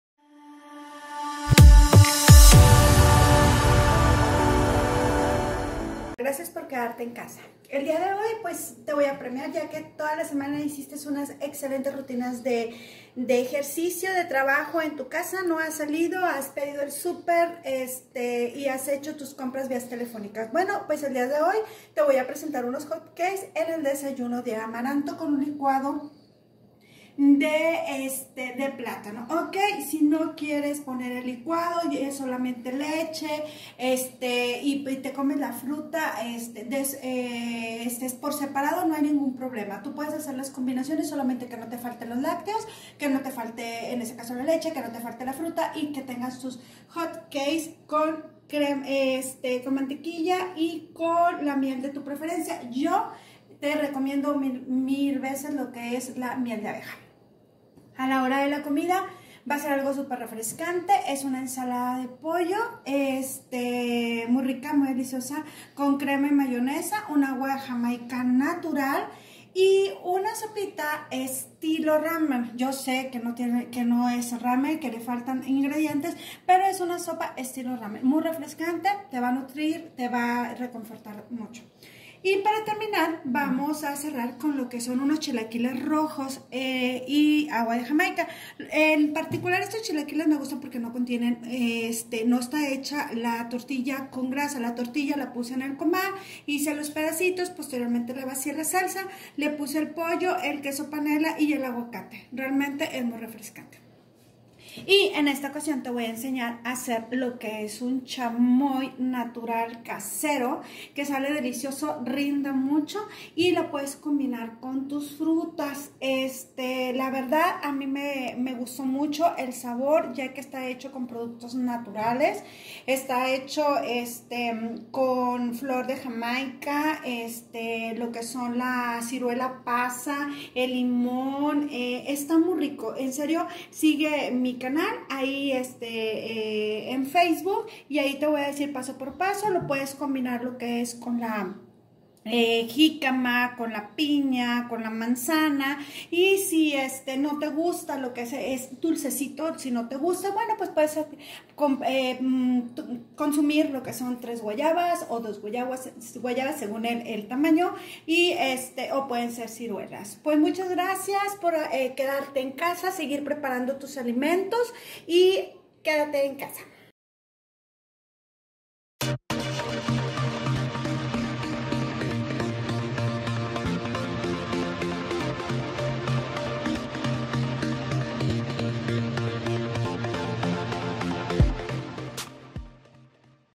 Menú día 9
La nutrióloga y chef Irisabella García comparte estos tips que te ayudarán en la creación de buenos hábitos de alimentación.